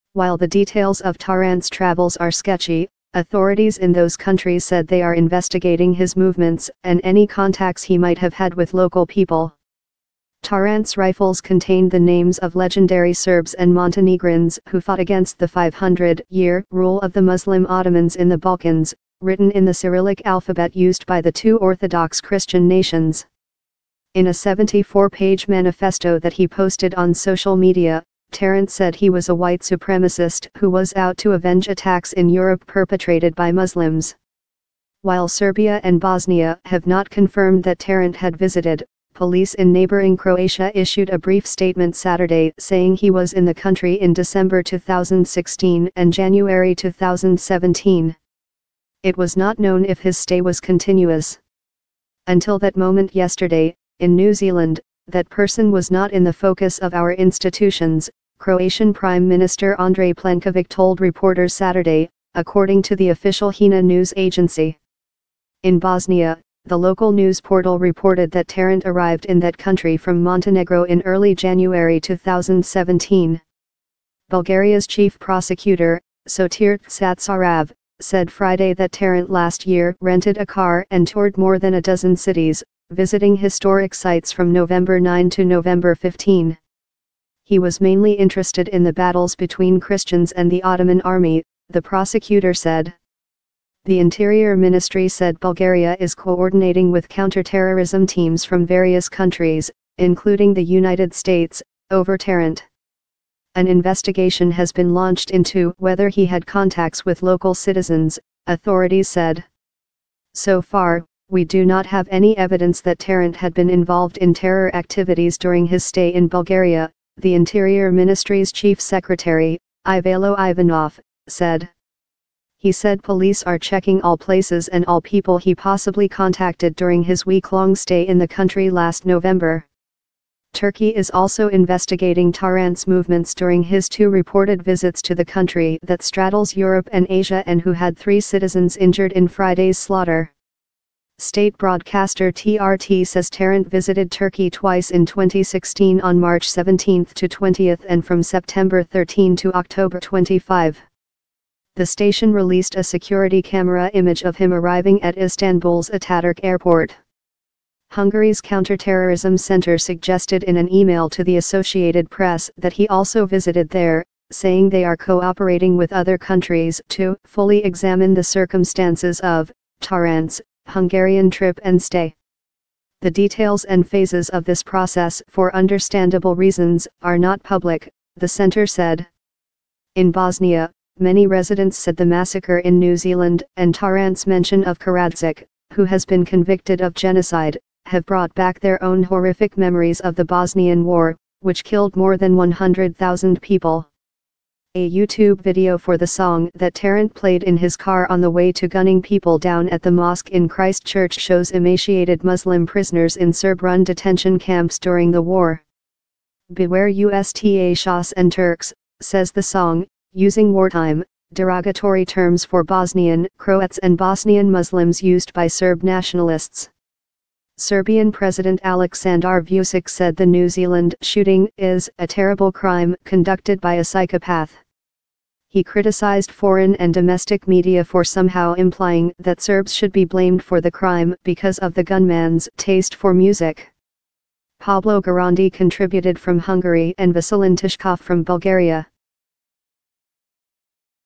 New Zealand Gunman Entranced With Ottoman Sites In Europe
While the details of Tarrants travels are sketchy, authorities in those countries said they are investigating his movements and any contacts he might have had with local people.